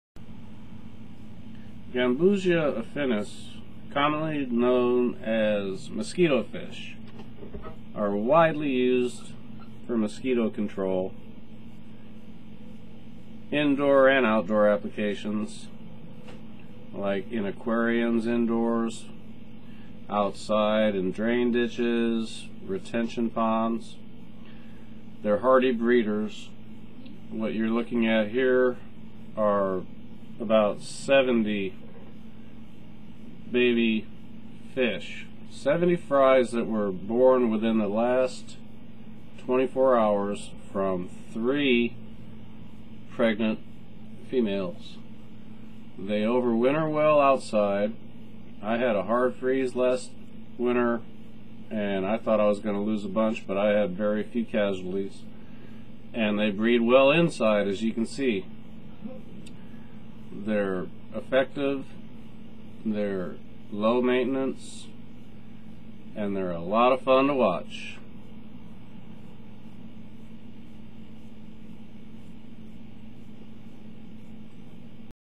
Gambusia Affinis (Mosquitofish) Babies
Gambusia Affinis, commonly known as mosquito fish, are widely used for mosquito control; indoor and outdoor applications.  Like in aquariums indoors, outside in drain ditches, retention ponds, etc...  They are hearty breeders.

What you are looking at here are about Seventy (70) baby fish.  Seventy fries that were born within the last 24 hours from 3 pregnant females.  They overwinter well outside.  I had a hard freeze last winter and I thought I was going to lose a bunch but I had very few casualties and they breed well inside as you can see.  They're effective, they're low maintenance and they're a lot of fun to watch.